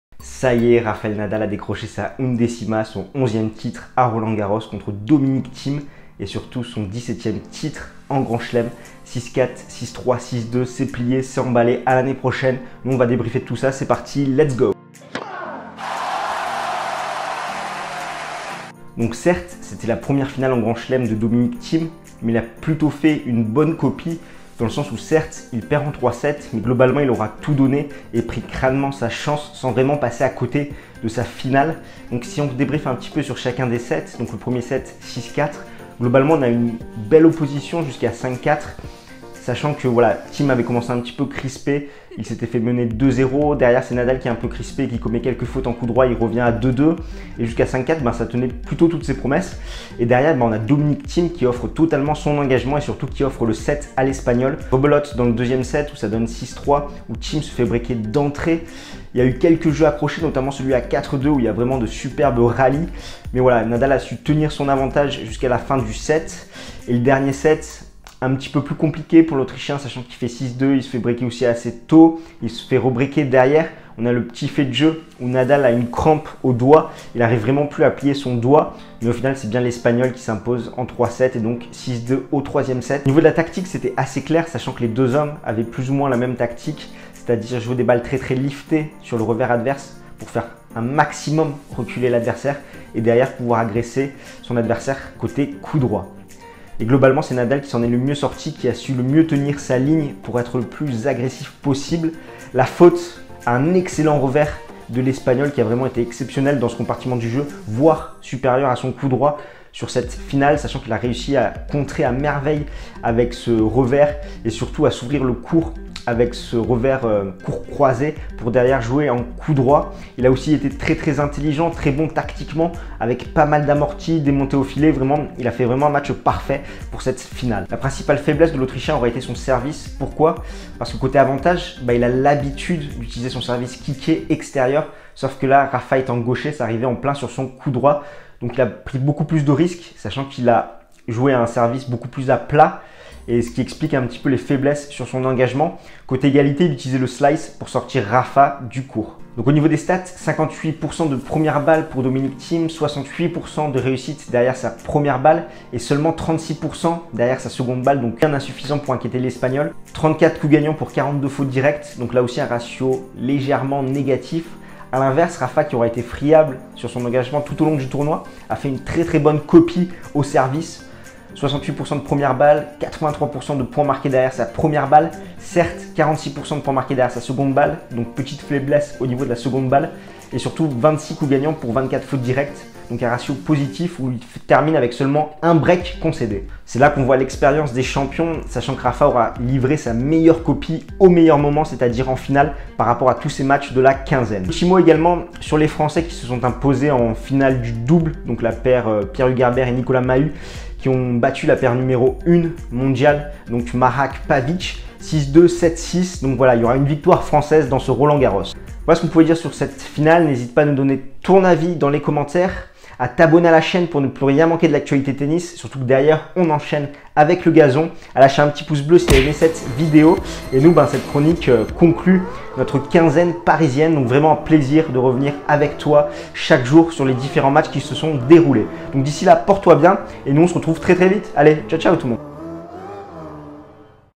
NADAL DÉCROCHE SA UNDÉCIMA CONTRE THIEM | RG18 - Jour 15
RAFAEL NADAL L'A FAIT ! Il décroche ainsi sa Undécima à Roland-Garros 2018 ! Son 11ème titre à Roland-Garros contre Dominic Thiem ! Et surtout son 17ème titre en Grand-Chelem ! Retour sur une finale qui s'est terminée en 3 sets !